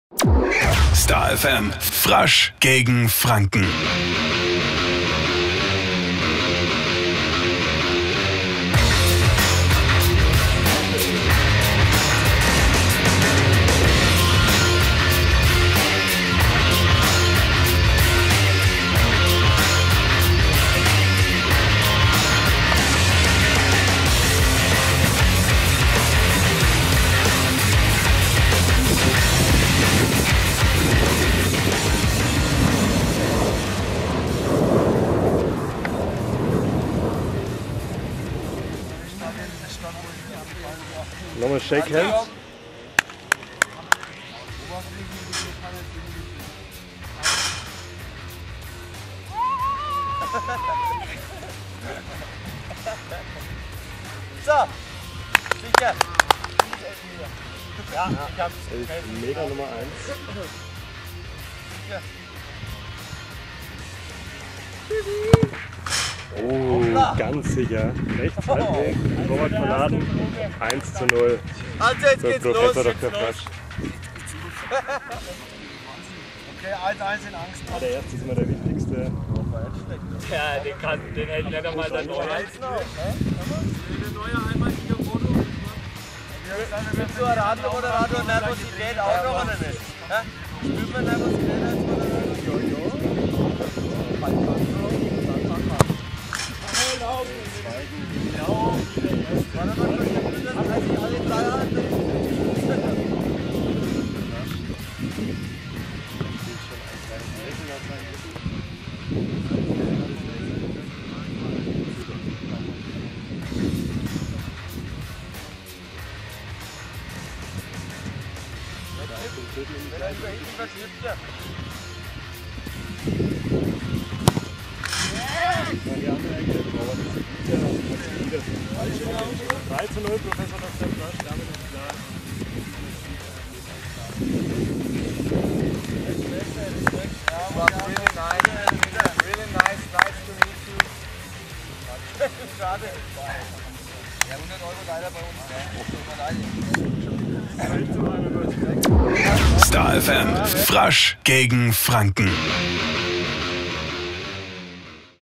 Frasch gegen Franken - Elfmeterduell gegen den TSV Buch
STAR FM Moderator Daniel Frasch tritt gegen fränkische Fußballvereine im Elfmeterschießen an. Mindestens drei von fünf Elfmetern müssen verwandelt werden,  sonst wandern 100 Euro in die Mannschaftskasse.